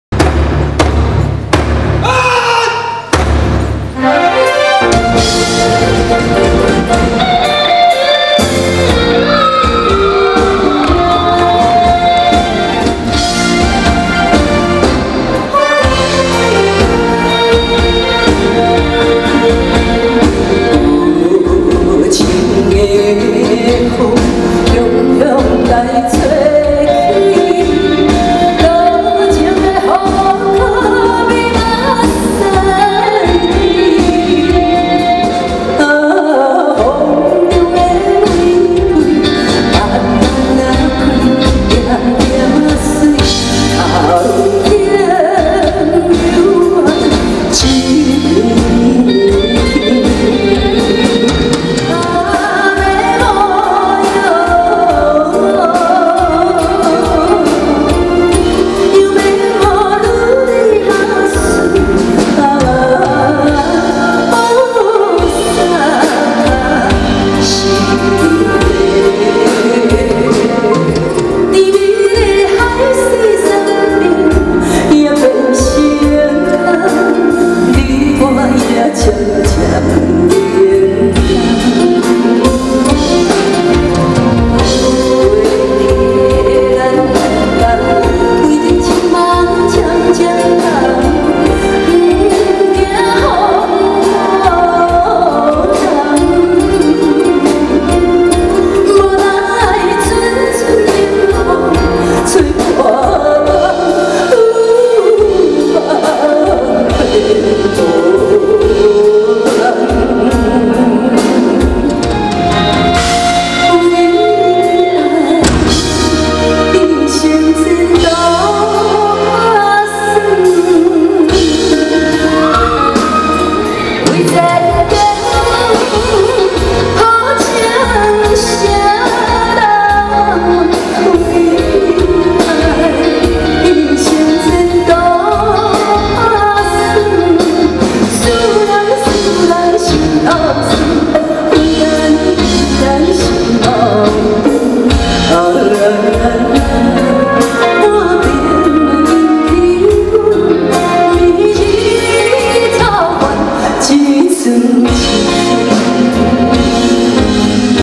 龍千玉 玩變裝秀《龍千玉 羅時豐 龍飛豐舞演唱會》
2014年最溫暖，最有愛的演唱會- 龍飛豐舞演唱會（龍千玉+羅時豐）6/7-6/8 於台北國際會議中心盛大演出！